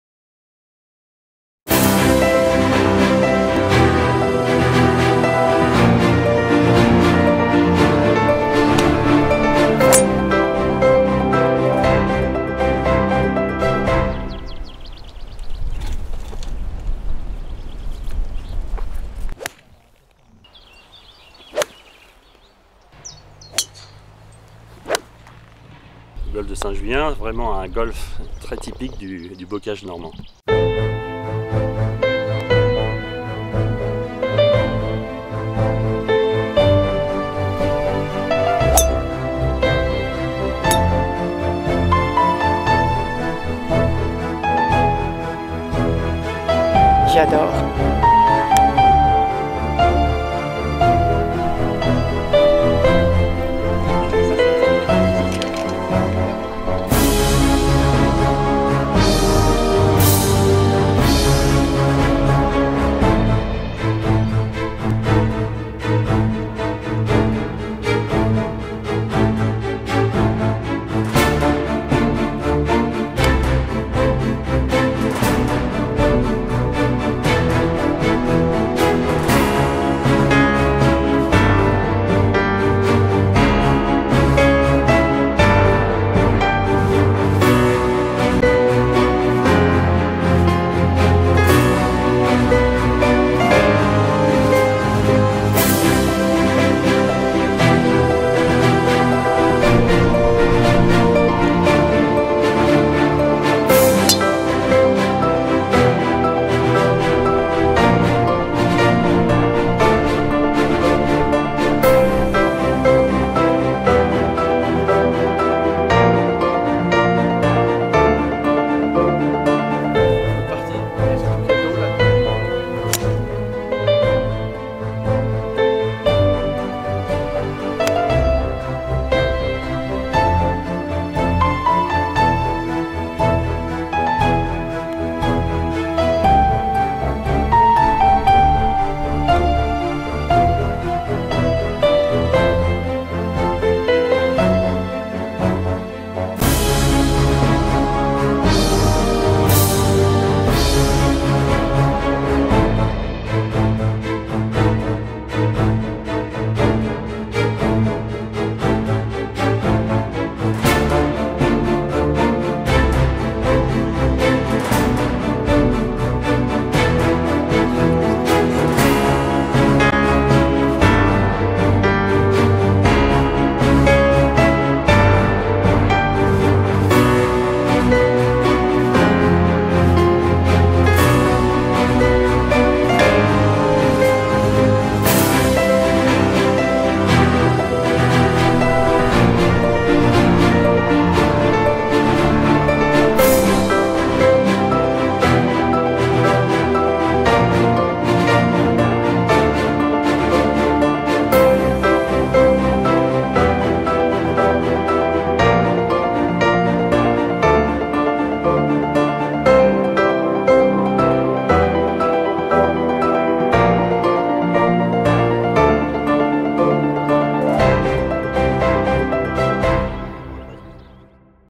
GROUPE BARRIERE - GOLF ST JULIEN
Le Golf St Julien fait partie des plus fameux golf de Normandie du Groupe Barrière.
Pour mieux le découvrir et en apprécier toutes les possibilités golfiques, Le groupe Barrière a souhaité confier à la société Atoutcam la réalisation de cette  vidéo promotionnelle : A vos clubs !